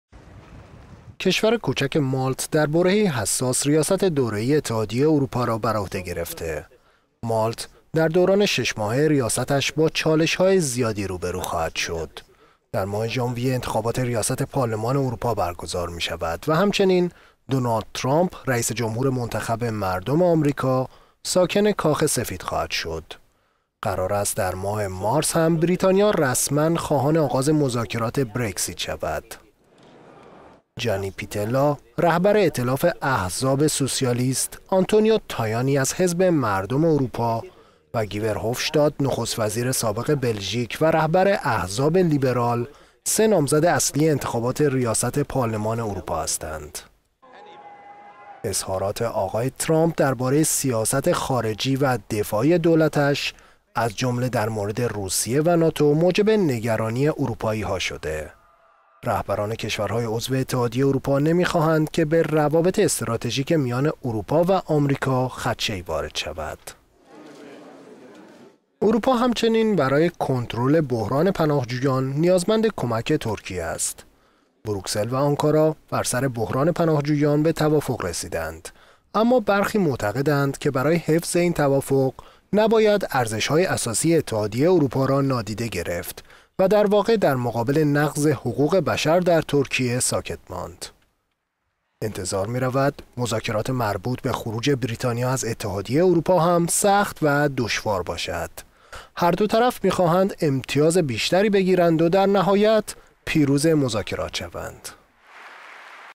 چالشهای اتحادیه اروپا در سال ۲۰۱۷
کشور کوچک مالت در برهه ای حساس ریاست دوره ای اتحادیه اروپا را برعهده گرفته است. مالت در دوران شش ماه ریاستش با چالشهای زیادی روبرو خواهد شد.

در ماه ژانویه، انتخابات ریاست پارلمان اروپا برگزار می شود و همچنین دونالد ترامپ، رئیس جمهور منتخب مردم آمریکا ساکن کاخ سفید خواهد شد. قرار است در ماه مارس هم بریتانیا رسما خواهان آغاز مذاکرات برکسیت شود.

جانی پیتلا، رهبر ائتلاف احزاب سوسیالیست، آنتونیو تایانی از حزب مردم اروپا و گی ورهوفشتات، نخست وزیر سابق بلژیک و رهبر احزاب لیبرال، سه نامزد اصلی انتخاب…